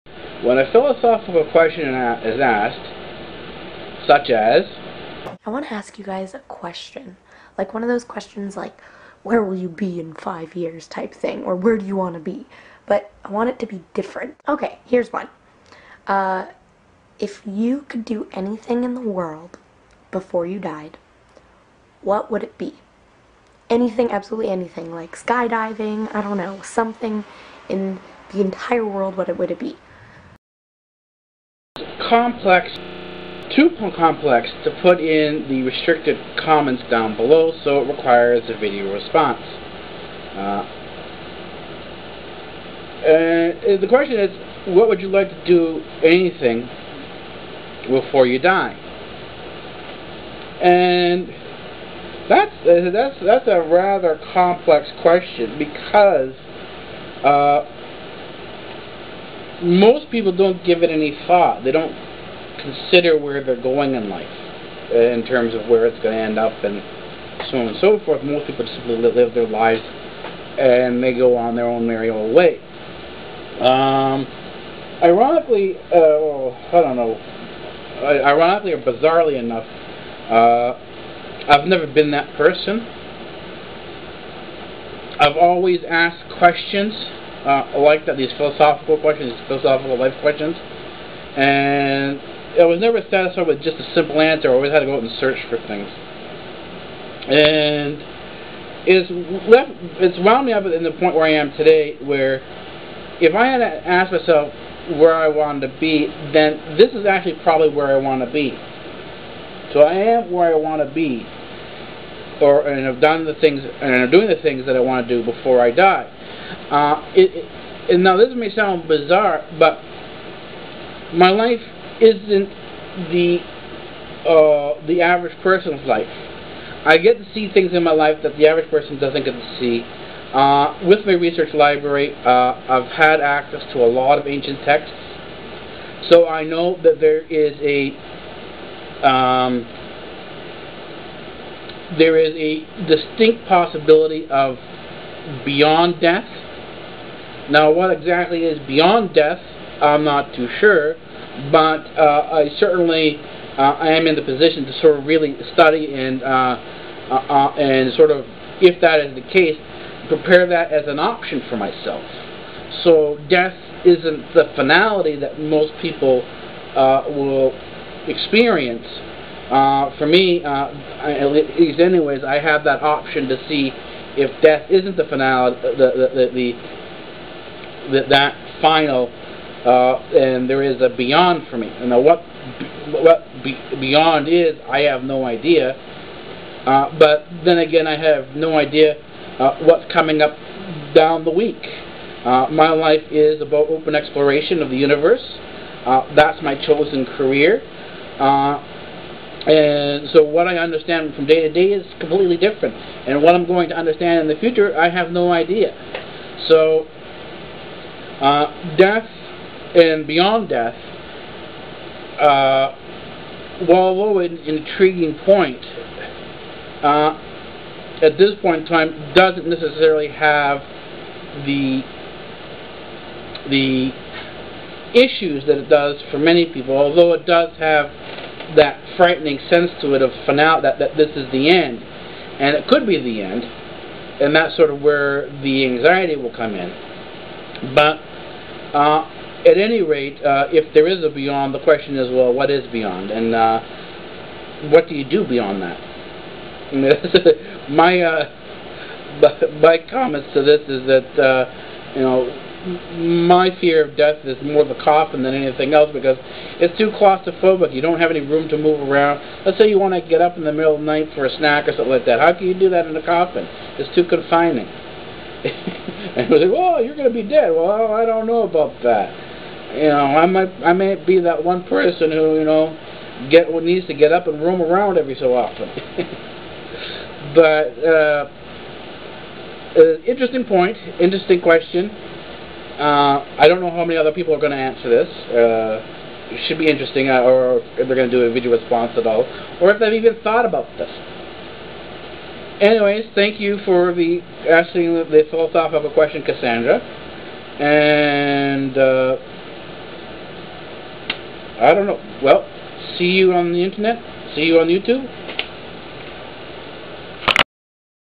Lifes Tough Questions - Comments Y - BigBangTheoryRL
Cassandra, of NerdzRL (in the video Jump for Joy ) asks one of those philosophical life assessing questions.